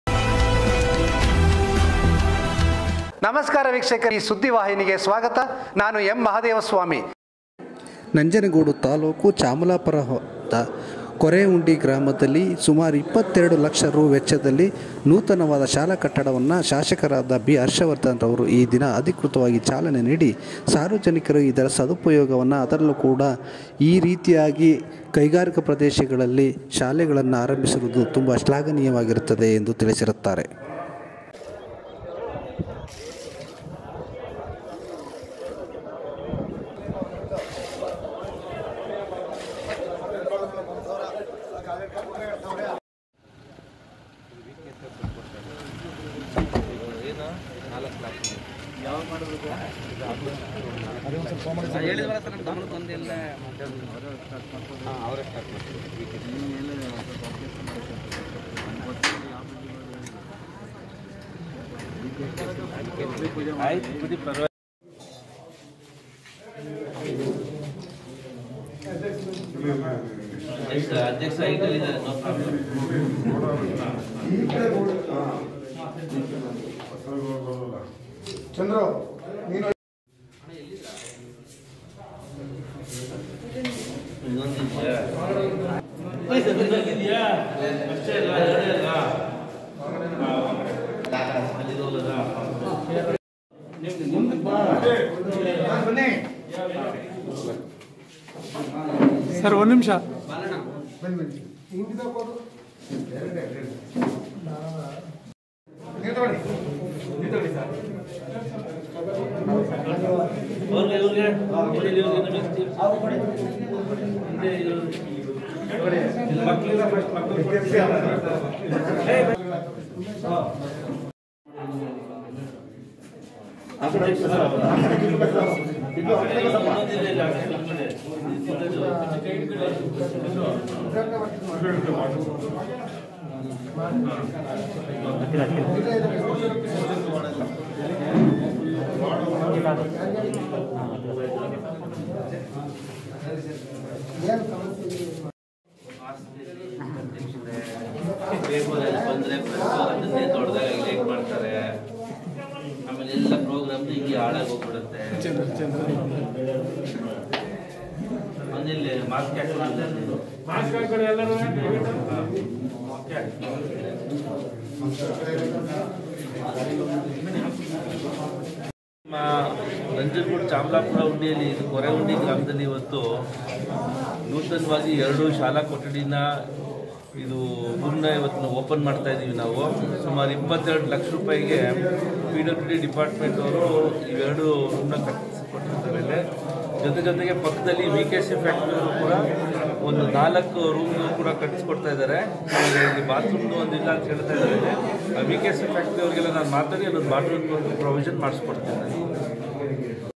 ನಂಜನಗೂಡು ತಾಲೂಕು ಚಾಮಲಾಪುರ ಹುಂಡಿಯ ಕೊರೆ ಹುಂಡಿ ಗ್ರಾಮದಲ್ಲಿ ನೂತನ ಶಾಲಾ ಕಟ್ಟಡ ಉದ್ಘಾಟನೆ
ನಂಜನಗೂಡು ತಾಲೂಕು ಚಾಮಲಾಪುರ ದ ಹುಂಡಿಯ ಕೊರೆ ಉಂಡಿ ಗ್ರಾಮದಲ್ಲಿ ನೂತನ ಶಾಲಾ ಕಟ್ಟಡವನ್ನು ಕ್ಷೇತ್ರದ ಶಾಸಕರಾದ ಬಿ ಹರ್ಷವರ್ಧನ್ ರವರು ಚಾಲನೆ ನೀಡಿರುತ್ತಾರೆ